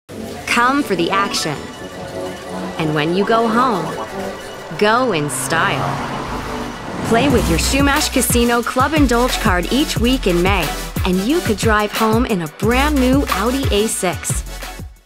May 2017 Audi A6 Giveaway
Play at Chumash Casino for your chance to win one of four car giveaways in May.